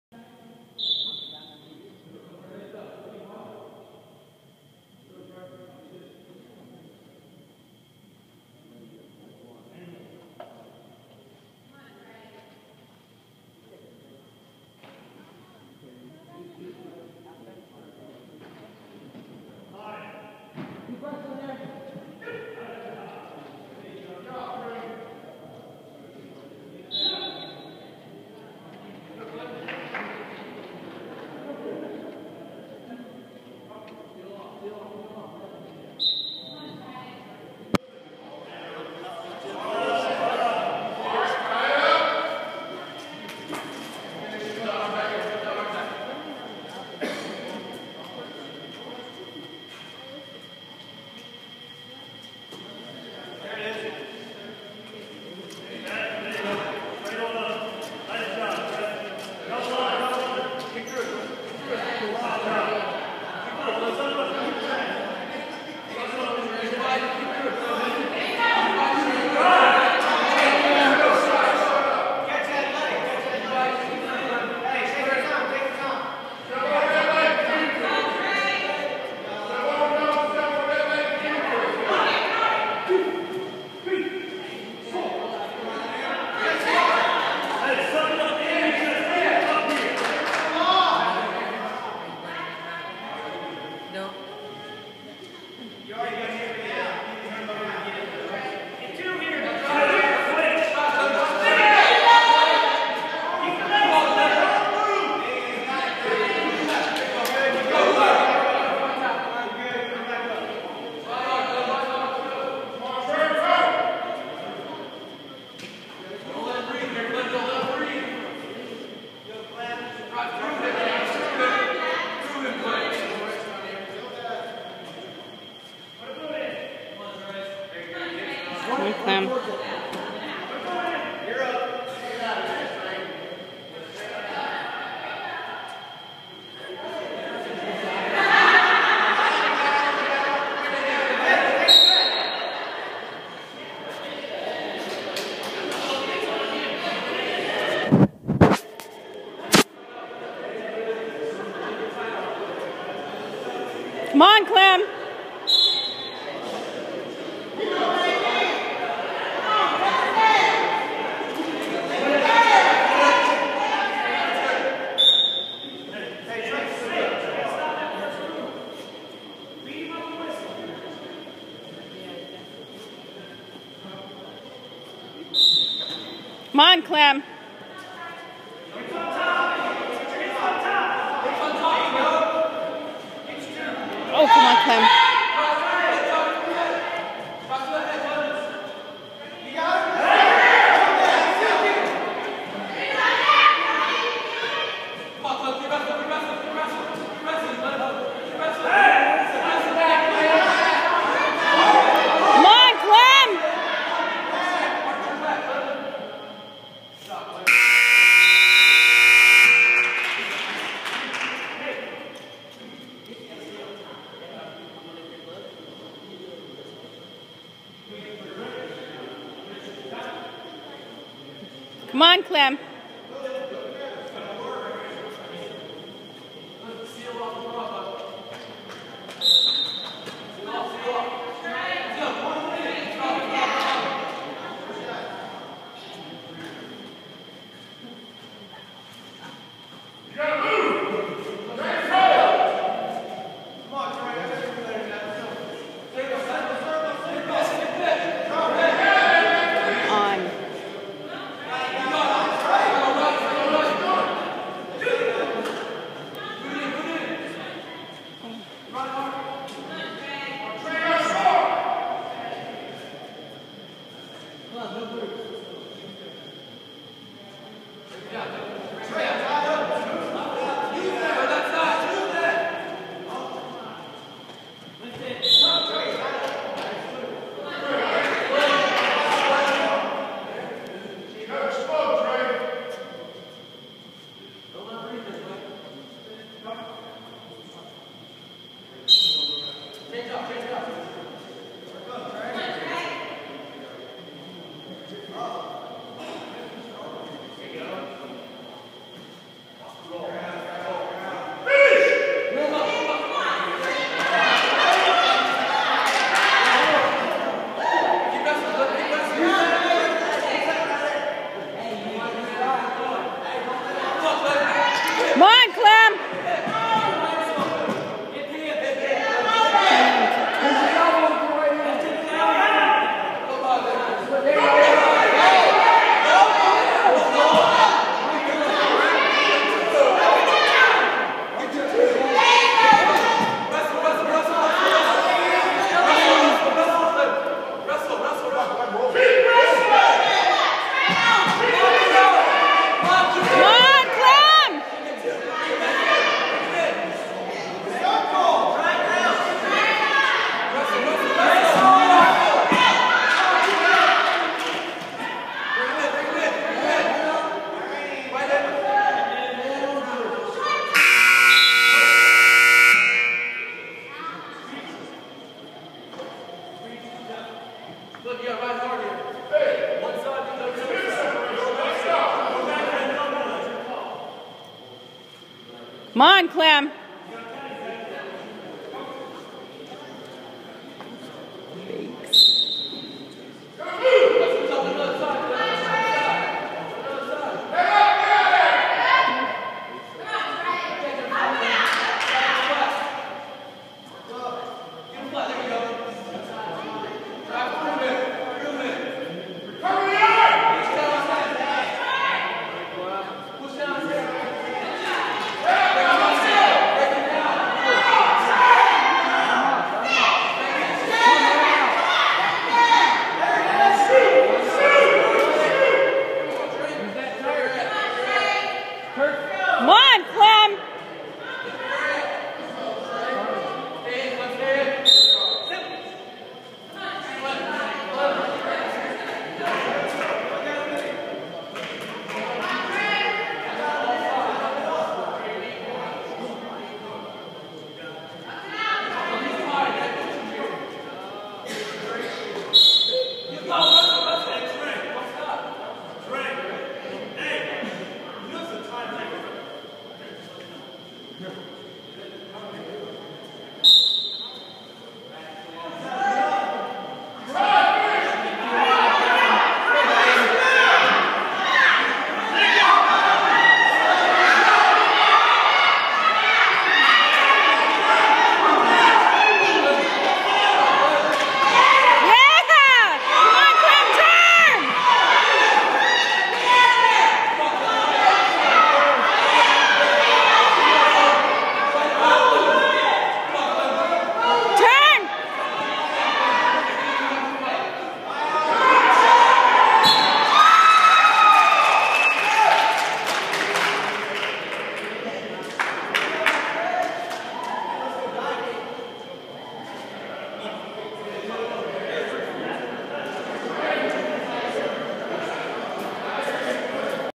Clem Narvaiz (BAC) vs Spartanburg Methodist college @ BAC
3rd match of 2012-13 year @ 149lbs loss fall  record 0-3